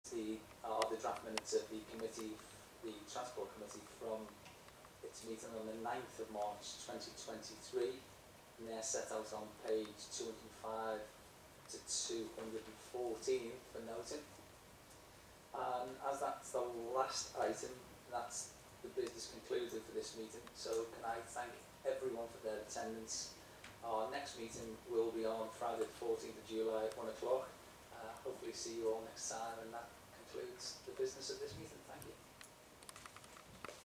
Liverpool City Region Combined Authority 9th June 2023 Part 3 of 3
Filmed by John Brace at Authority Chamber, First Floor, 1 Mann Island, Liverpool L3 1BP.

The agenda and reports for this public meeting of Liverpool City Region Combined Authority can be found

20. Draft Minutes of the Transport Committee held on 9 March 2023 0:00